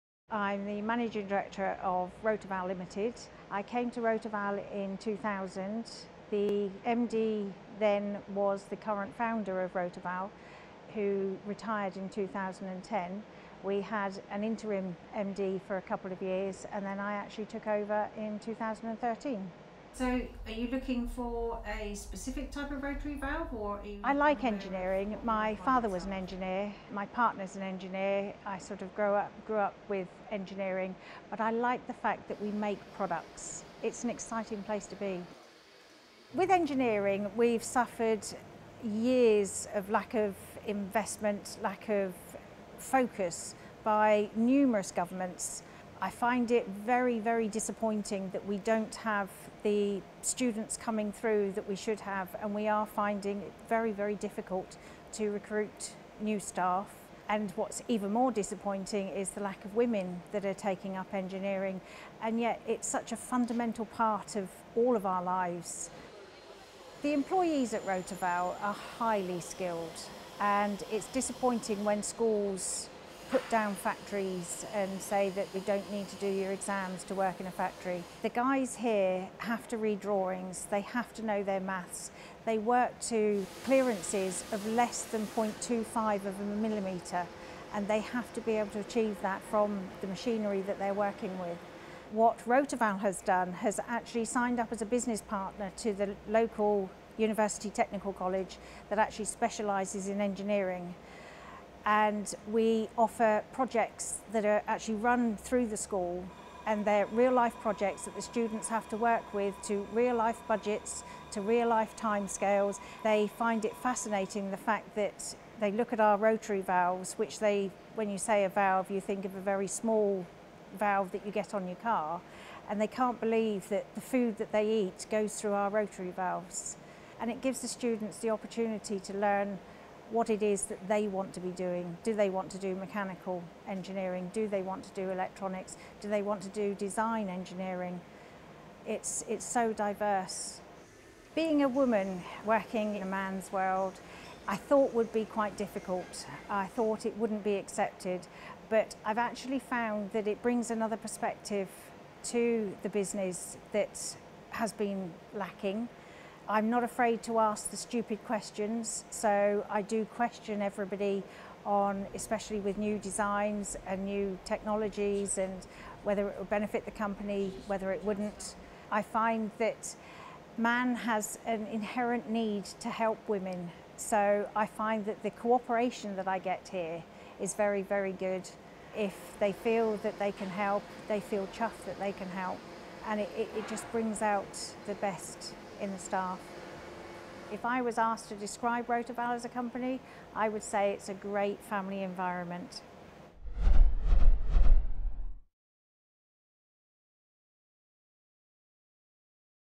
Gericke RotaVal Beverley Ford -Women in Engineering
Beverley Ford of Gericke RotaVal speaking about Women in Engineering